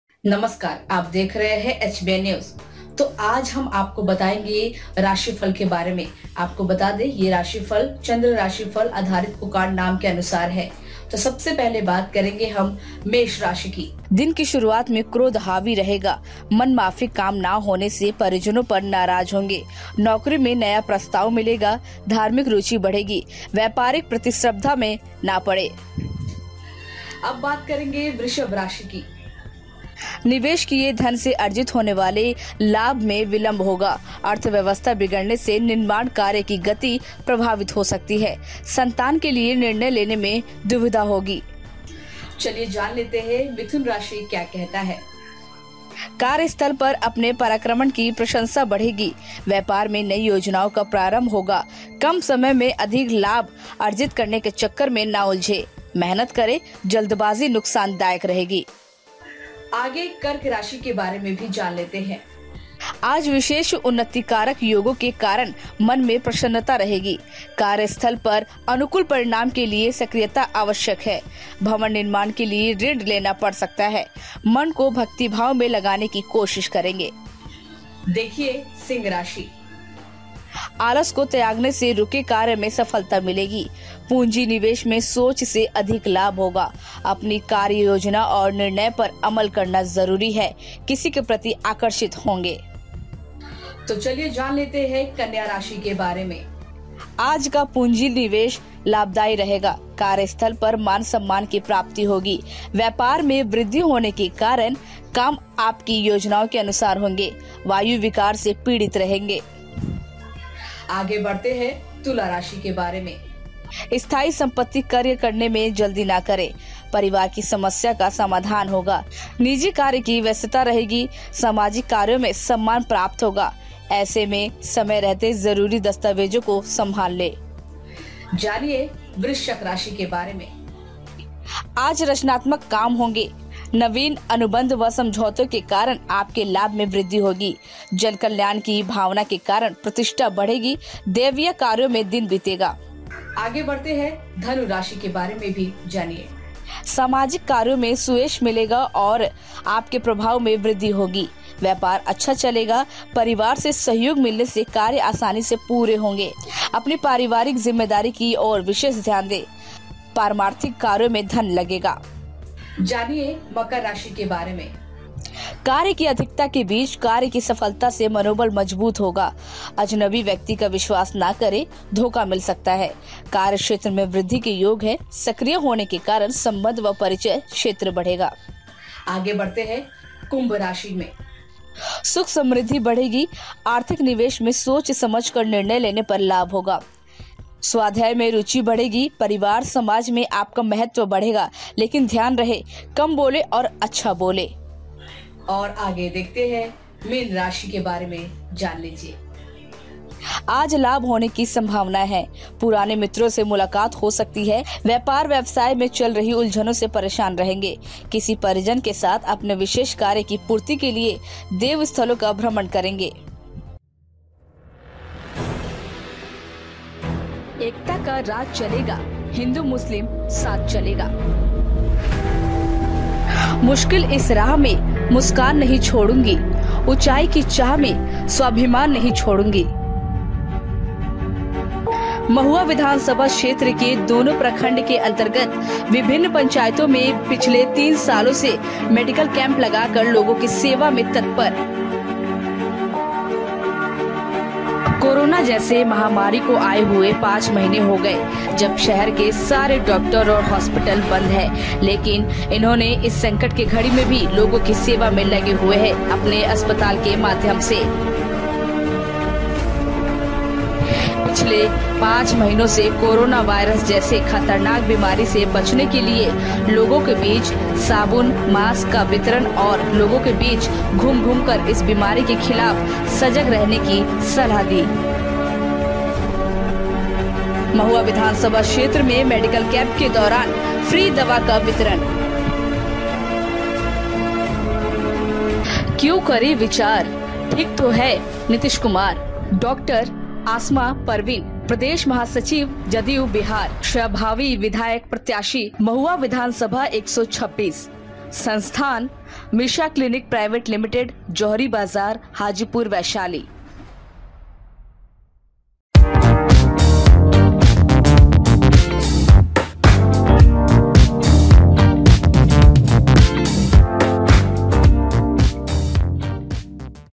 जाने अपनी राशि के बारे में कुछ रोमांचित बाते | HBN News
मधुबनी,बिहार से पंकज कुमार कर्ण के साथ पंकज झा शास्त्री

05/08/2020,बुधवार।

यह राशि फल चंद्र राशि अधारित पुकार नाम अनुसार है।यह कोइ जरुरी नही कि सभी शब्द किसी एक व्यक्ति विशेष पर मिल ही जाए।अतः बेहतर परिणाम जानने हेतु जन्म कुंडली का अध्यन जरुरी होता हैं।